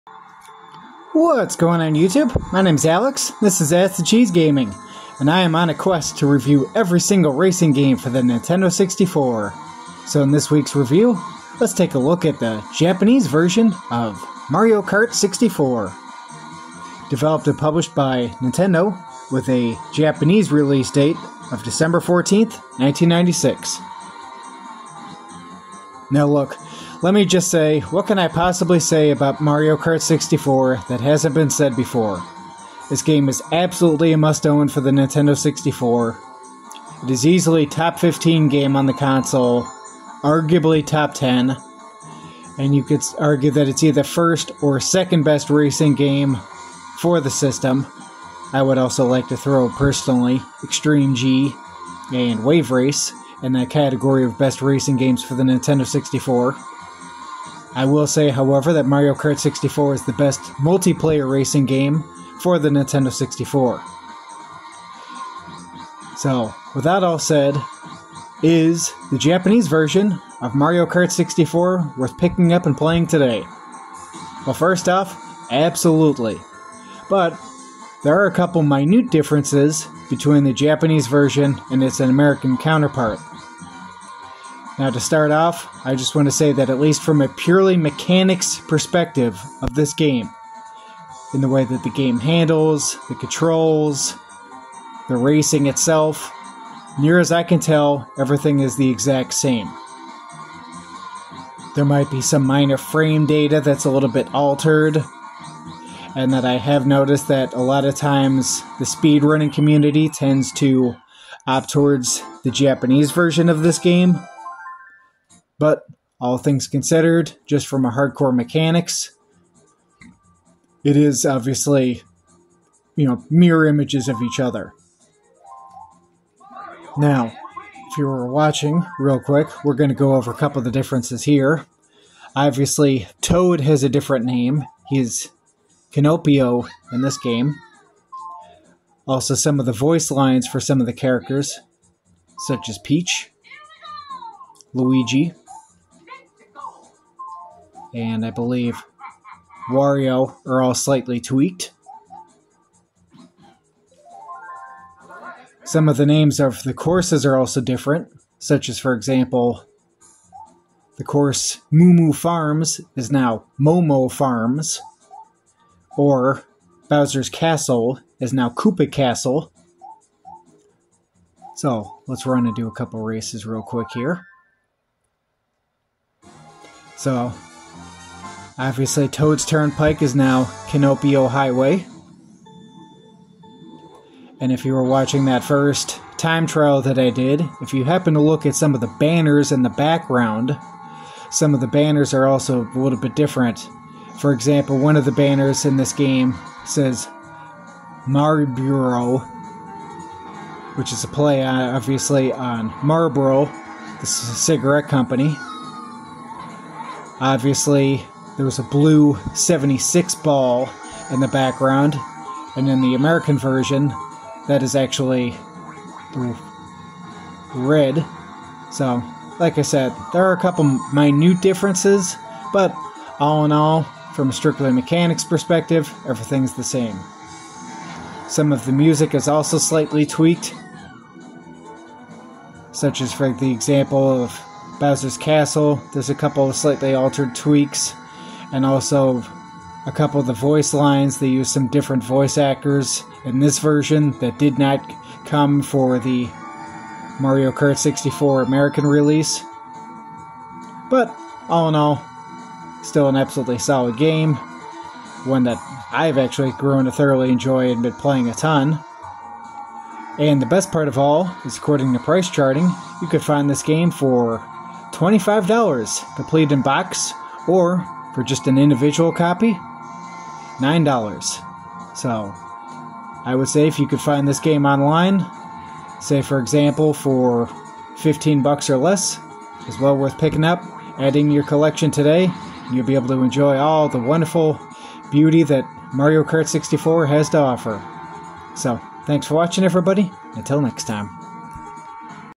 JP Mario Kart game review
Obviously Mario kart 64 is a must own for the nintendo64games but what about its Japanese counter part ?